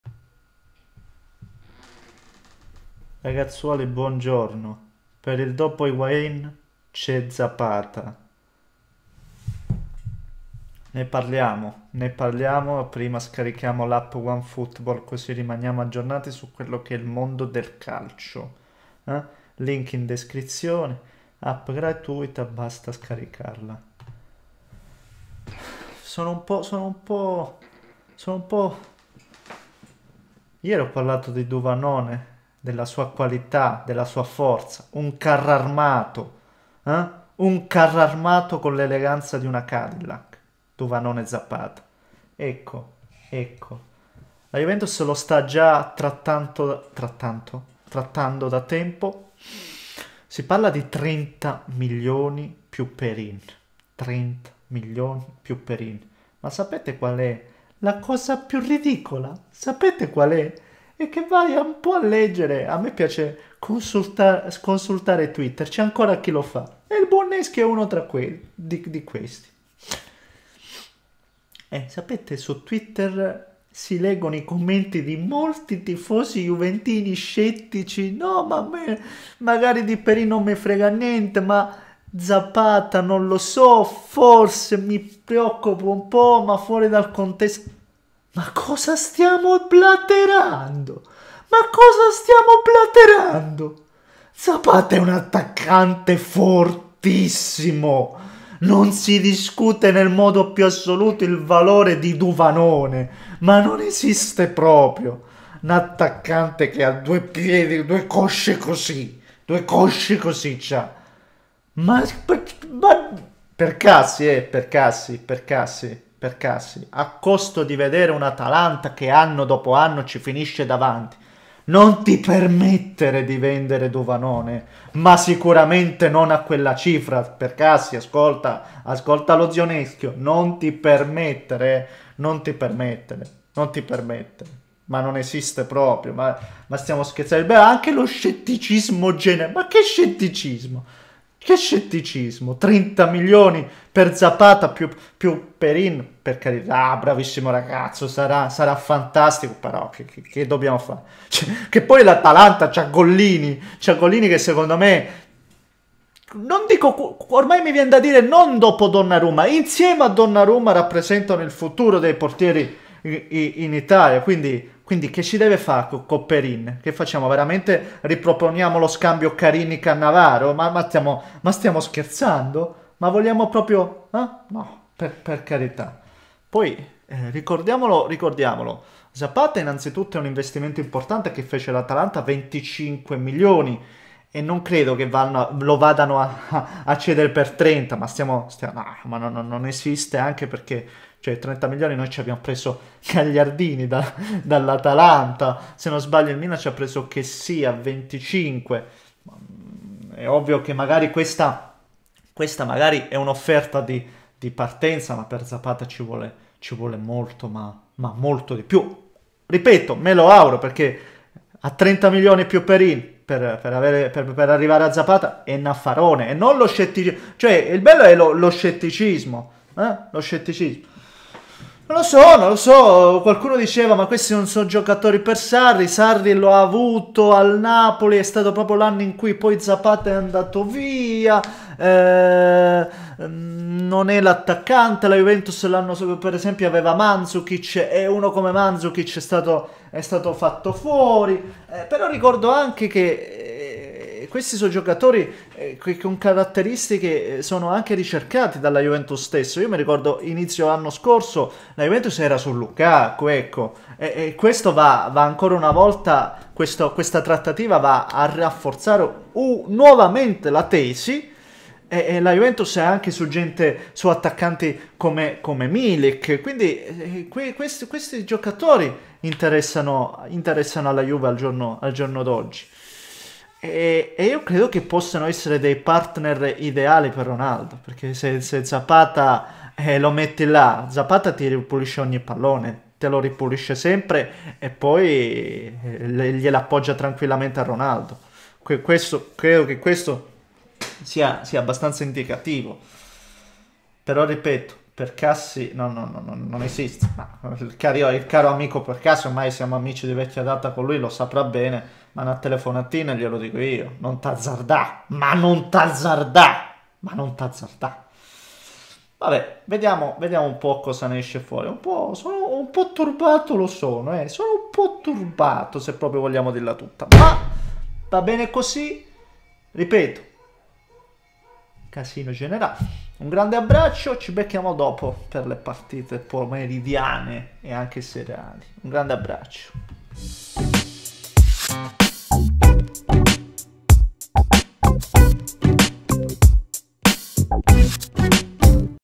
[SONO TURBATO] LA JUVE STA TRATTANDO ZAPATA | Perin + 30 milioni per Duvanone!
✅ SCARICA ONEFOOTBALL

La Juve forte su Zapata
Paratici ha sviluppato la trattativa con l’Atalanta per il centravanti colombiano: Perin e 30 milioni a Bergamo, in uscita Gollini. Il dopo-Higuaìn è Duvàn che lasciò Napoli poiché chiuso dal Pipita
CORRIERE DELLO SPORT

‼️ SEGUITEMI...

✅ PER COLLABORAZIONI...